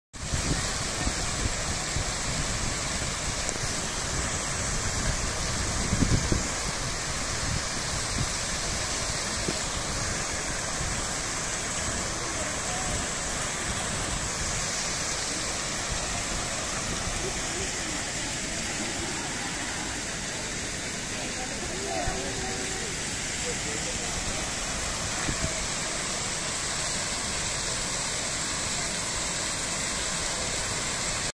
Fuente Gefion Gefionspringvandet
Fuente junto a la iglesia anglicana de San Albano (Sankt Albans Kirke) Copenhague